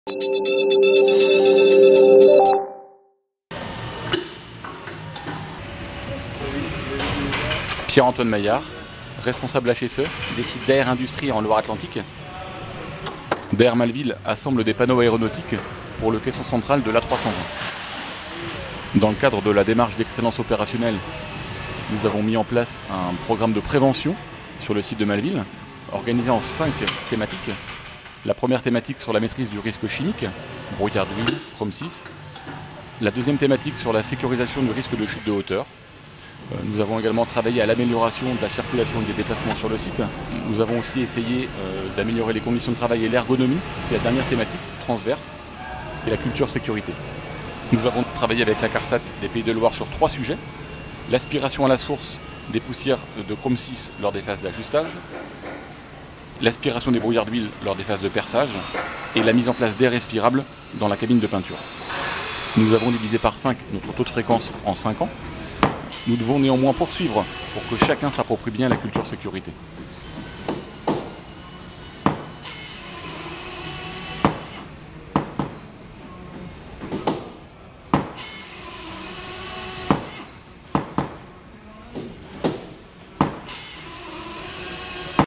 Sur le terrain - Démarche de prévention dans l'aéronautique - Daher, site de Malville
Diaporama sonore réalisé par l'équipe de Travail & Sécurité. Il présente la politique de prévention du site de Malville de l'entreprise Daher, sous-traitant informatique. Et en particulier le travail réalisé avec la Carsat Pays de la Loire sur de nombreux risques, et notamment le risque chimique.

Référence INRS : Anim-239